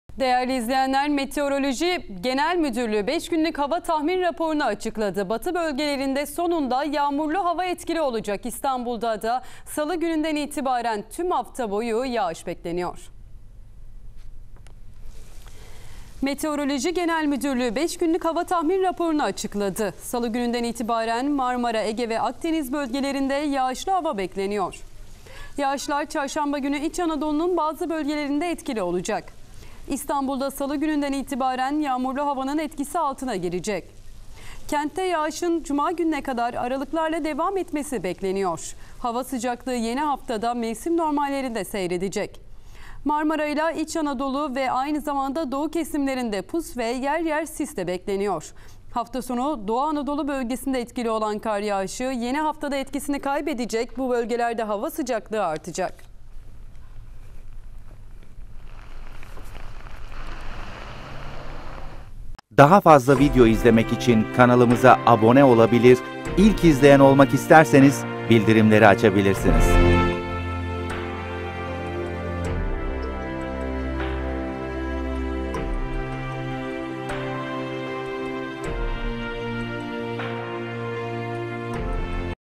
Salı Günü İçin Kritik Uyarı! İstanbullular Dikkat
Meteoroloji Genel Müdürlüğü,  salı gününden itibaren Marmara, Ege ve Akdeniz bölgelerinde yağış beklendiğini açıkladı. Yağışlar, çarşamba günü İç Anadolu'nun bazı bölgelerinde de etkili olacak.